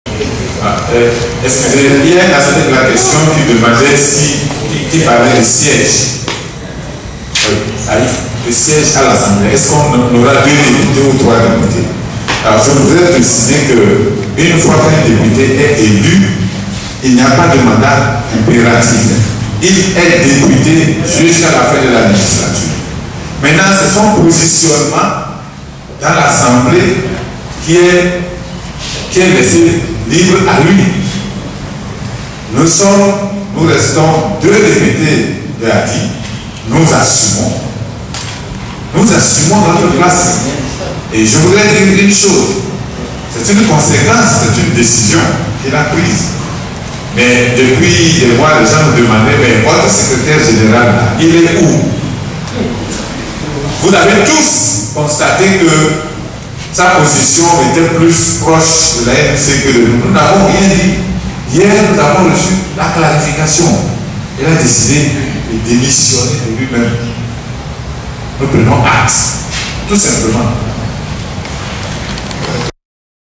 Dr François KAMPATIBE "Nous restons deux députés d'ADDI, nous assumons notre place. "
06 Novembre 2014. Docteur François KAMPATIBE répond aux questions des journalistes lors  de la conférence de presse que le nouveau couple politique  ADDI/CAR  a organisée au CESAL pour informer la presse de l'initiative prise par certains députés d'introduire un nouveau projet de loi portant révision de la constitution.